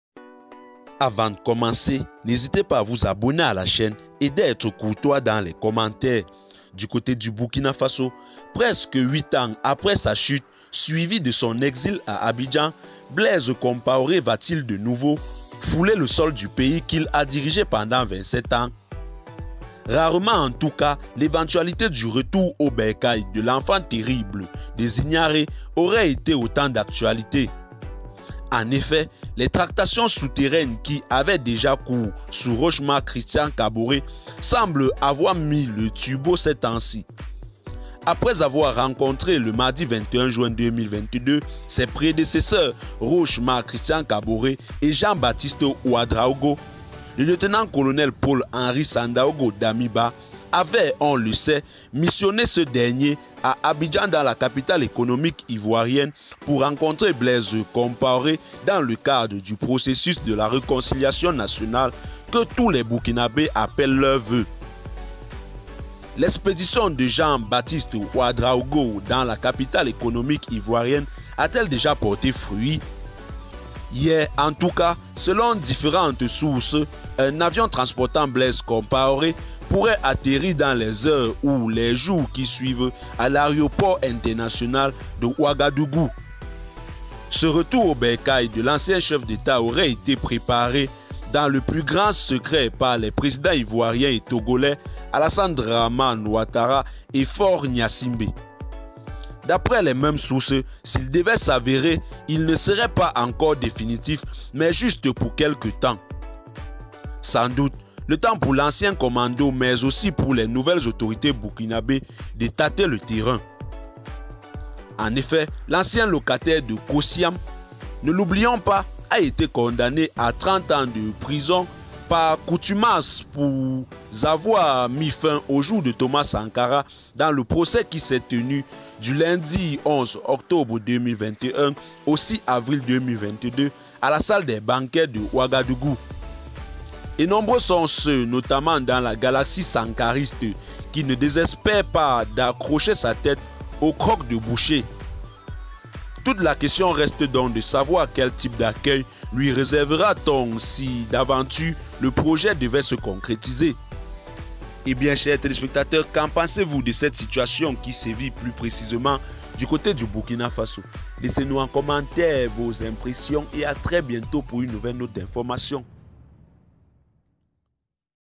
tout Ouagadougou dehors pour acclamé le retour triomphal de Blaise Compaoré...!
l'arrivée de Blaise Compaoré au Burkina Faso ! ce jour à réveiller le peuple burkinabè